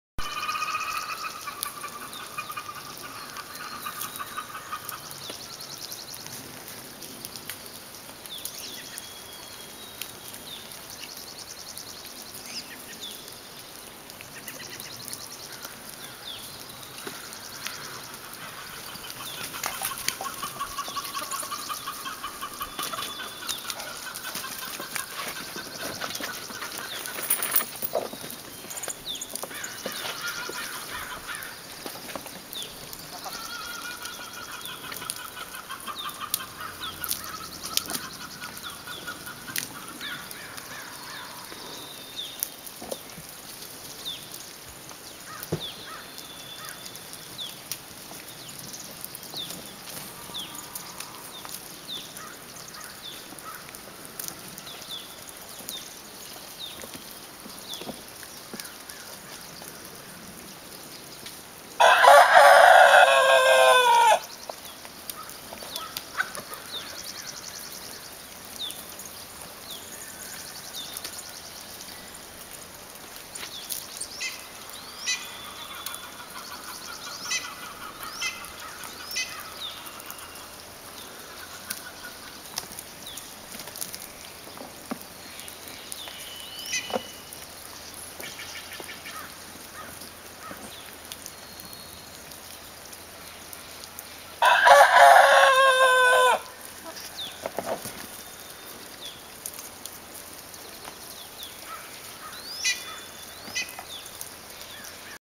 Lou... da' man!
Our Rooster Lou trying to impress his girls (and most especially Betty, the other Blue Orp) with his prowess.